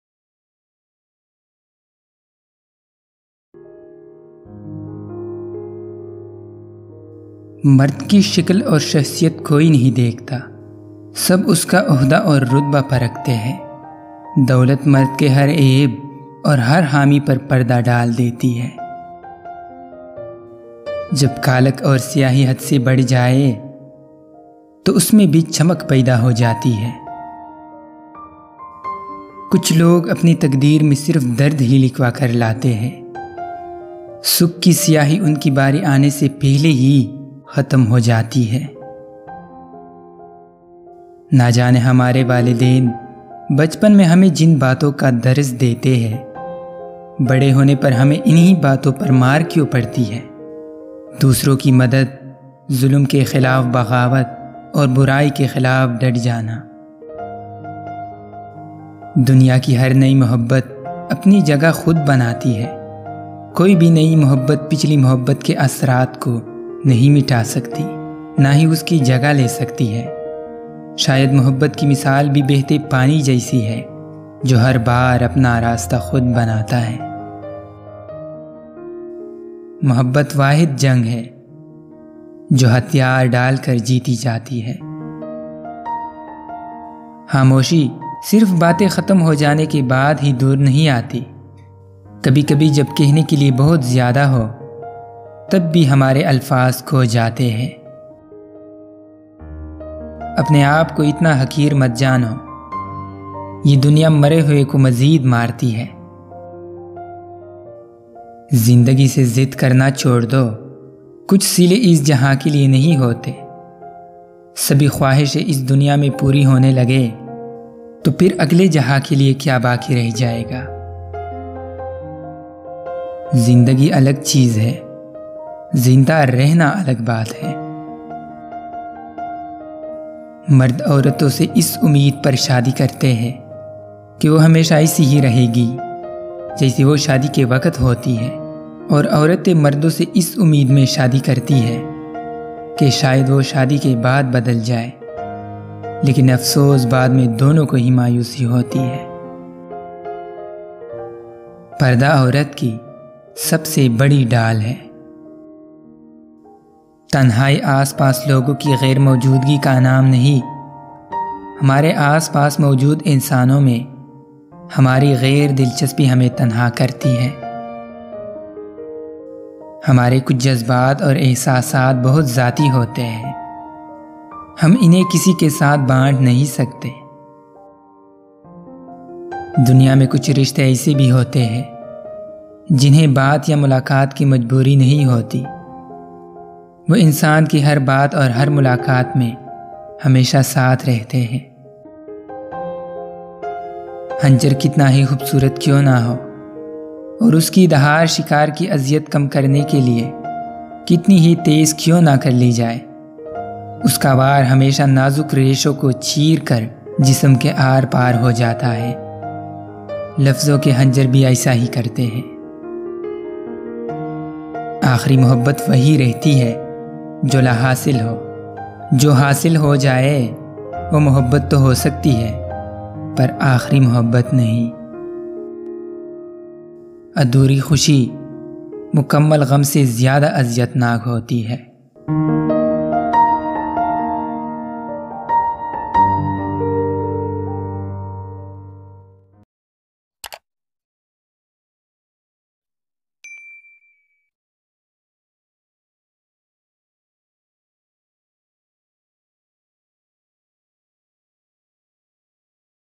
HASHIM NADEEM QUOTES | best collection of quotes | Knowledge.com
in this video some of beautiful Questions by famous Pakistani Novelist, Poet, Film producer and Writer "HASHIM NADEEM" Quotes in Urdu Language.